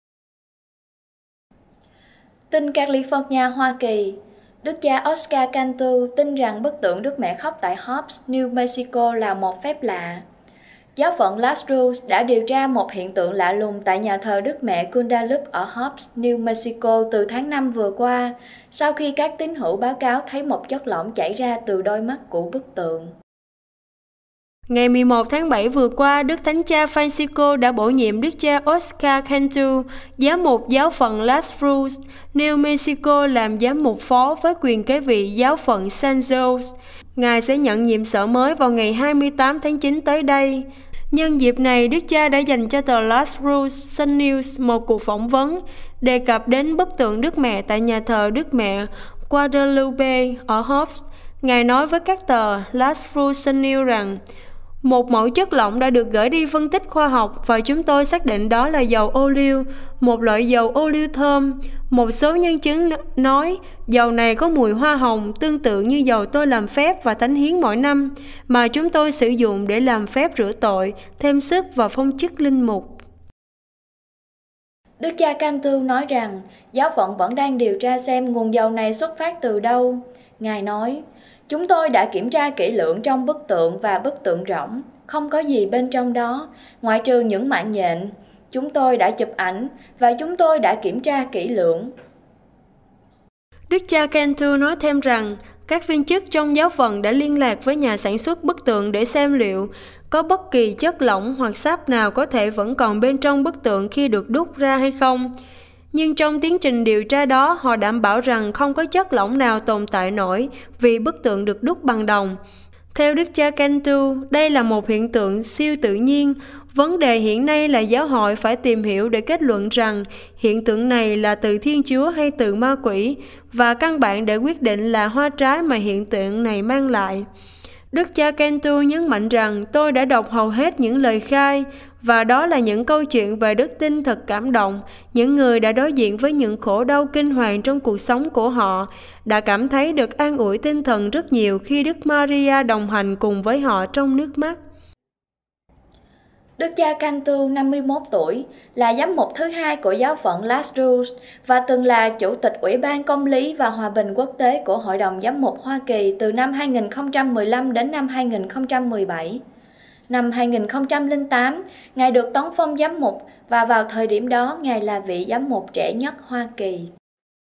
Đức Cha Oscar Cantú tin rằng bức tượng Đức Mẹ khóc tại Hobbs, New Mexico là một phép lạ
TRÍCH: GIÁO HỘI HOÀN VŨ TUẦN QUA: 16.7 ĐẾN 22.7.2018